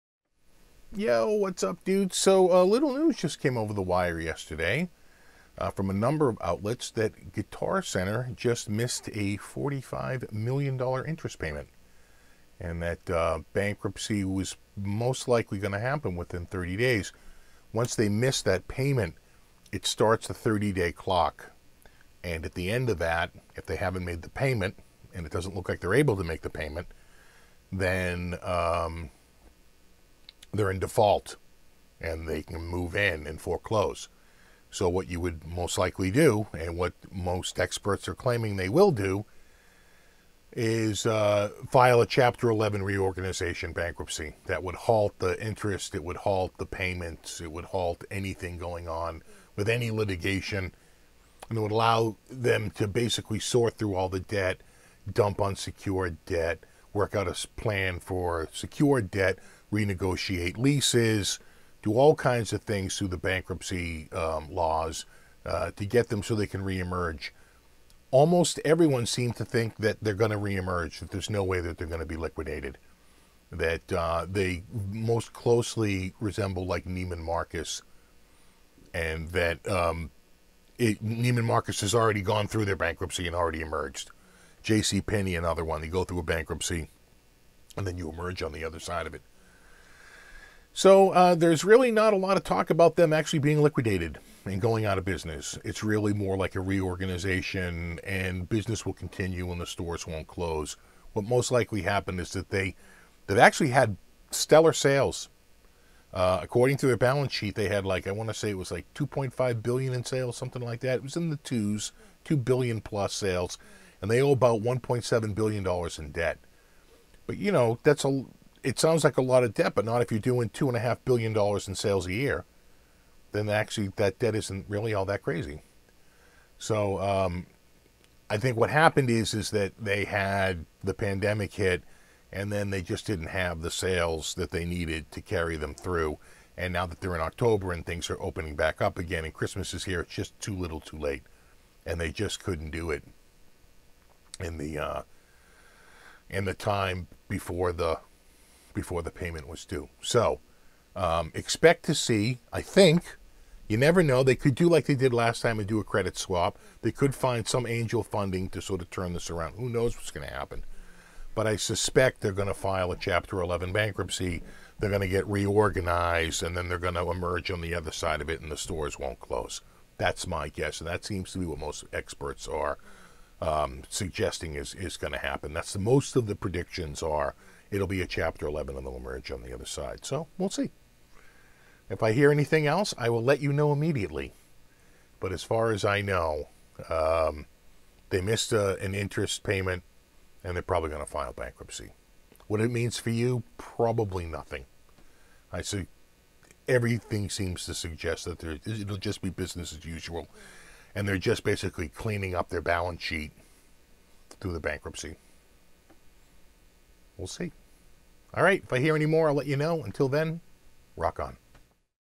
Guitar Center Misses $45m Interest Payment - Expected to File Bankruptcy Within 30 Days
I know we've seen this story before, but this time it may be for real.